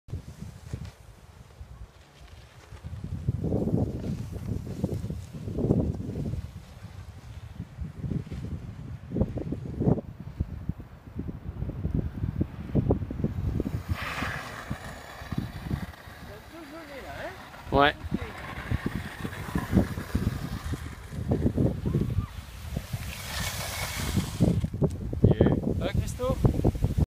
Aude Handisport - Tandem Ski - 27 janvier 2015
Descente en Tandem Ski lors du 1er séjour Handiski de la saison 2015 organisé par le Comité Handisport de l'Aude.